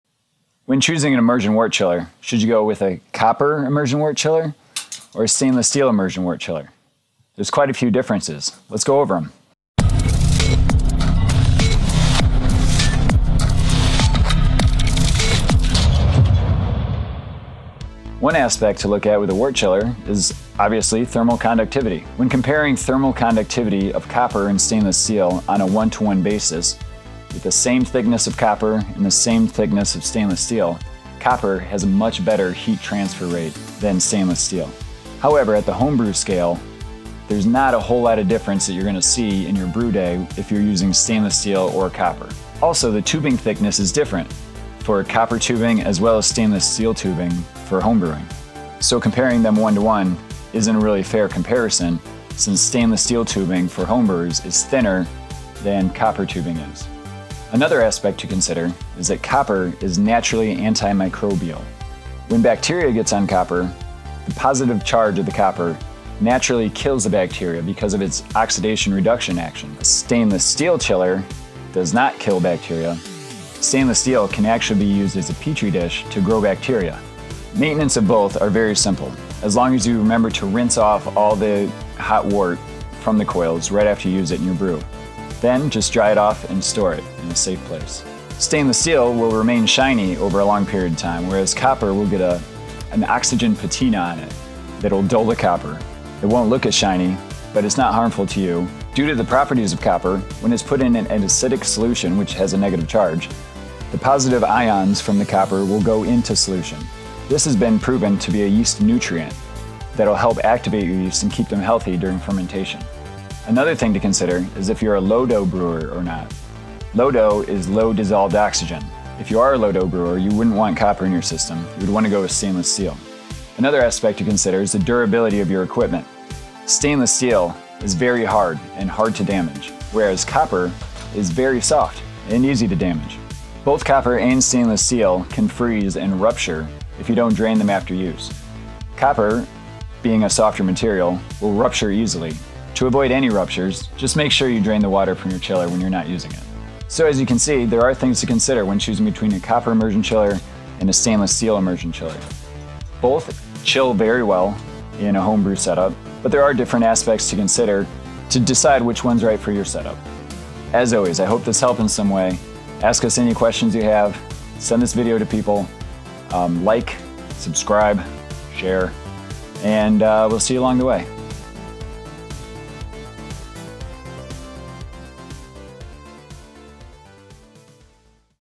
Copper vs Stainless Steel: Which Immersion Chiller is Best?
In this video, we're discussing pros and cons of copper immersion chillers as well as stainless steel immersion chillers to help you make the best choice for your homebrewing setup. Whether you're a seasoned brewer or just starting out, understanding the differences between copper and stainless steel immersion chillers can help you achieve the perfect brew!